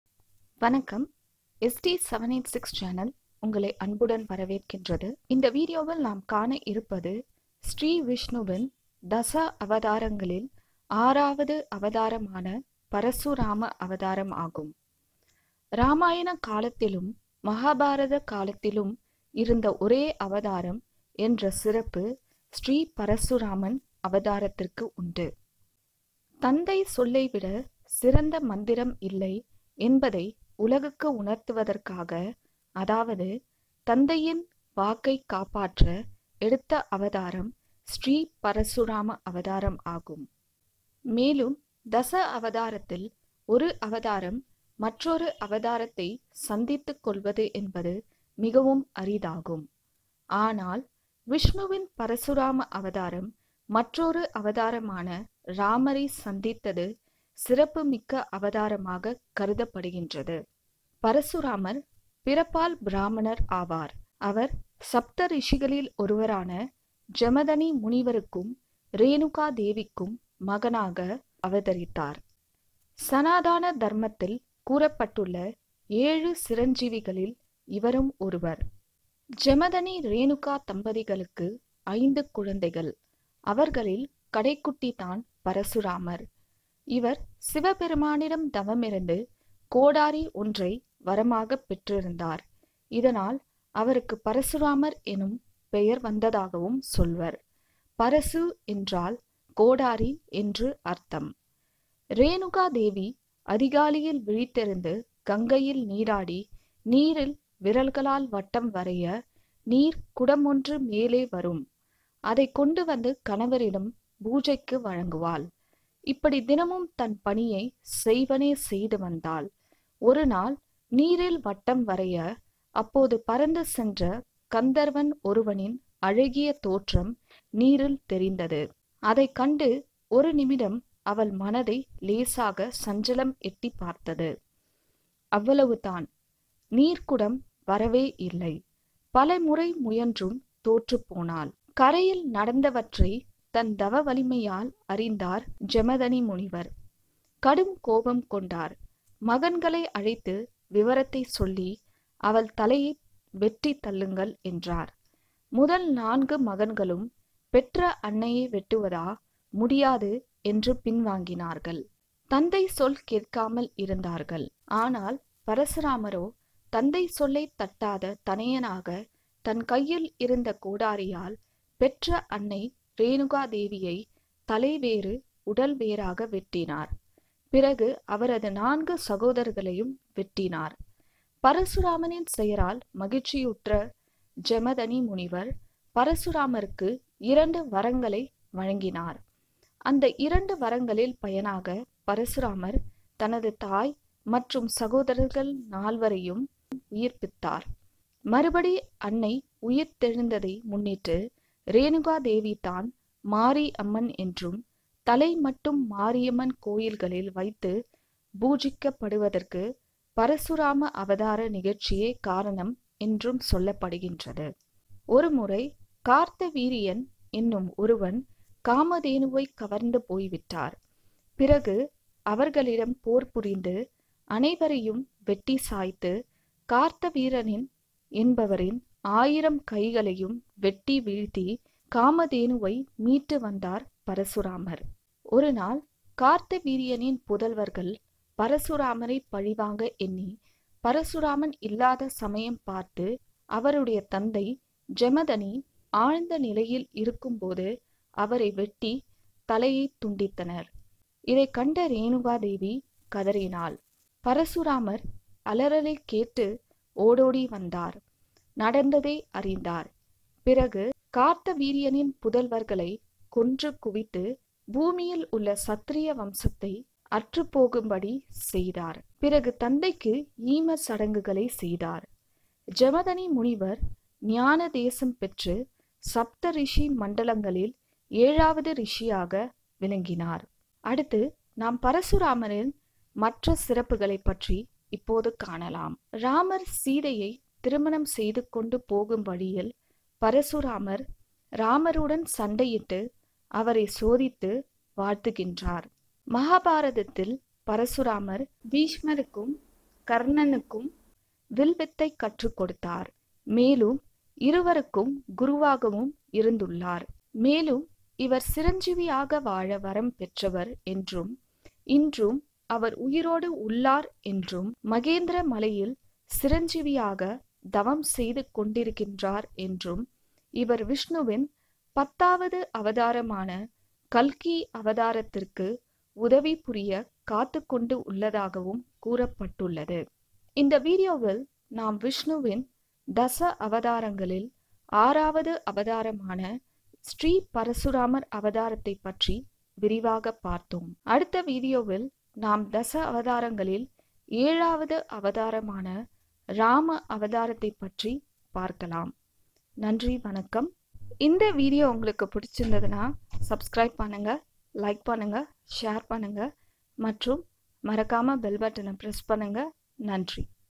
விஷ்ணுவின் தசாவதாரத்தின் ஆறாம் அவதாரமான "பரசுராமர் அவதாரம்" மற்றும் மகிமைகள்
இந்த வீடியோவில் நாம் விஷ்ணுவின் தசாவதாரத்தின் ஆறாம் அவதாரமான "பரசுராமர் அவதாரம்" மற்றும் மகிமைகள் பற்றி தெரிந்து கொள்ளலாம்.